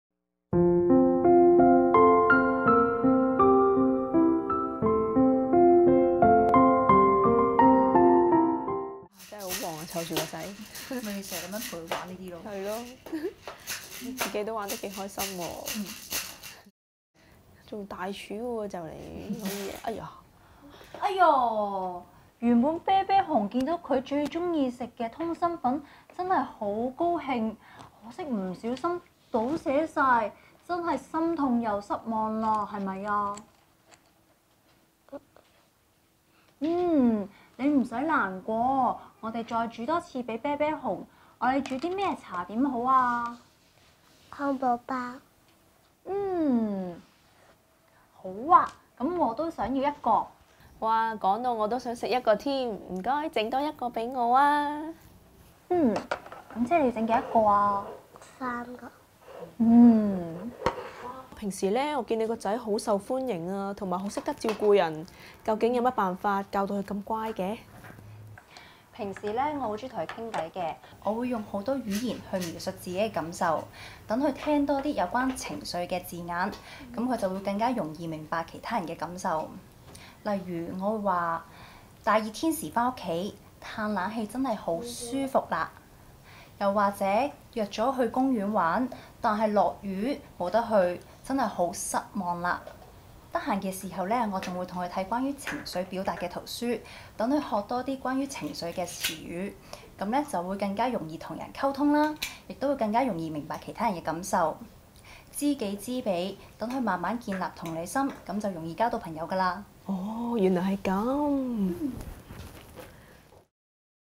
社交_建立同理心_汉堡包
家长多些认同孩子的感受，让孩子学习有关情绪的字词，有助孩子建立同理心。